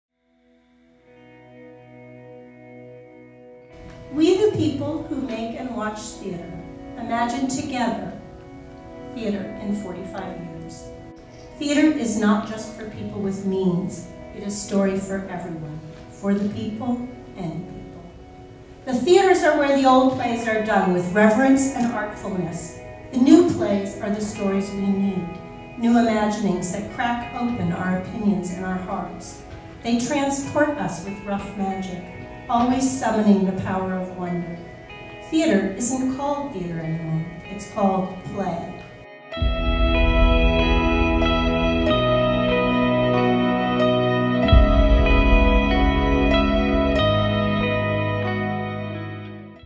Kira Obolensky on theater in 45 years | Give to the Max Day 2016
Imagine theater forward on Give to the Max Day, November 17, 2016. Schedule your Give to the Max Day donation to the Playwrights' Center in advance